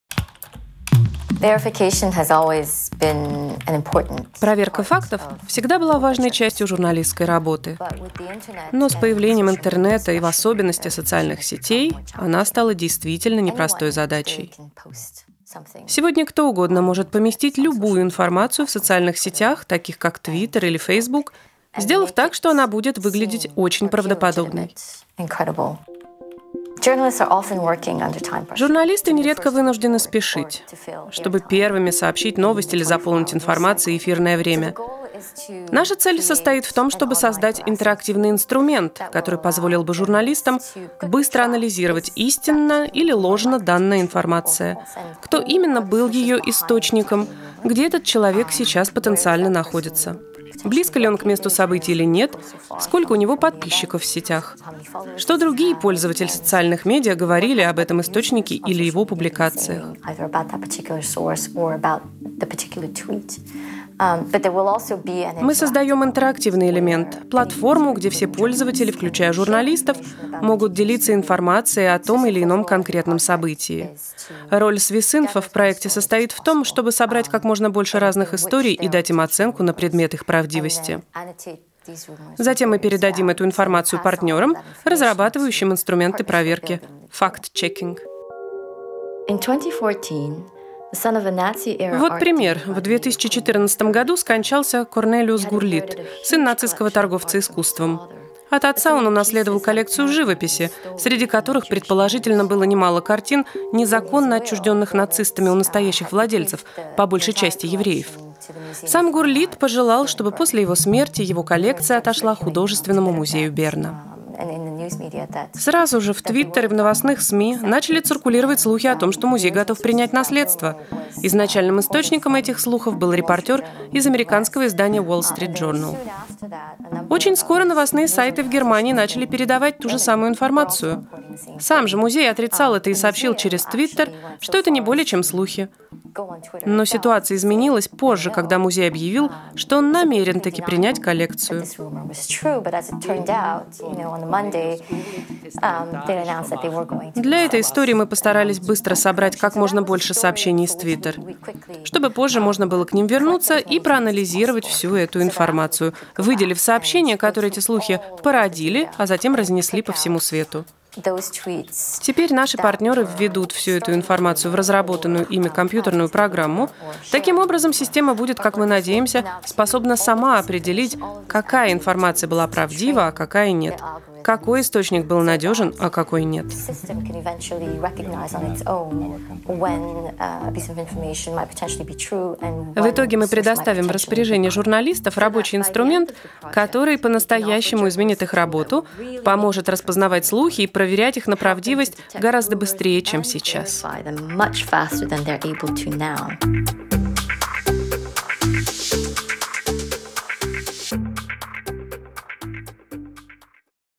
Фейк или правда? Как проверить информацию из соцсетей?
Как проверить, фейк или нет распространяется в сети?  Мы создаем интерактивный инструмент, который позволит журналистам быстро анализировать, истинна или ложна данная информация, кто именно был её источником, где этот человек сейчас потенциально находится, близко ли он к месту событий или нет, сколько у него подписчиков в сетях, что другие пользователи социальных медиа говорили об этом источнике или его публикациях.
Мы создаем интерактивный элемент, платформу, где все пользователи, включая журналистов, могут делиться информацией о том или ином конкретном событии. Роль swissinfo.ch в проекте состоит в том, чтобы собрать как можно больше разных историй и дать им оценку на предмет их правдивости. Затем мы передадим эту информацию партнерам, разрабатывающим инструменты проверки (фактчекинг).
что она будет выглядеть очень правдоподобной. Журналисты нередко вынуждены спешить,
чтобы первыми сообщить новость или заполнить информацией эфирное время.
Вот пример. В 2014 году скончался Корнелиус Гурлитт, сын нацистского торговца искусством. От отца он унаследовал коллекцию живописи, среди которых предположительно было немало картин, незаконно отчужденных нацистами у настоящих владельцев, по большей части евреев.
Сам Гурлитт пожелал, чтобы после его смерти его коллекция отошла Художественному музею Берна. Сразу же в Twitter и в новостных СМИ начали циркулировать слухи о том,
что музей готов принять наследство. Изначальным источником этих слухов был репортер
из американского издания Wall Street Journal.
Очень скоро новостные сайты в Германии начали передавать ту же самую информацию. Сам же музей отрицал это и сообщил через Тwitter, что это не более, чем слухи. Но ситуация изменилась позже, когда музей объявил, что он намерен-таки принять коллекцию. Для этой истории мы постарались быстро собрать как можно больше сообщений из Twitter, чтобы позже можно было к ним вернуться и проанализировать всю эту информацию, выделив сообщения, которые эти слухи породили, а затем разнесли по всему свету. Теперь наши партнеры введут всю эту информацию в разработанную ими компьютерную программу, таким образом система будет, как мы надеемся, способна сама определить, какая информация была правдива, а какая нет, какой источник был надежен, а какой нет!
В итоге мы предоставим в распоряжение журналистов рабочий инструмент, который по-настоящему изменит их работу, поможет распознавать слухи и проверять их на правдивость гораздо быстрее, чем сейчас.
Швейцарский новостной и информационный портал SWI swissinfo.ch входит в состав самой большой медийной корпорации в Швейцарии - SRG SSR.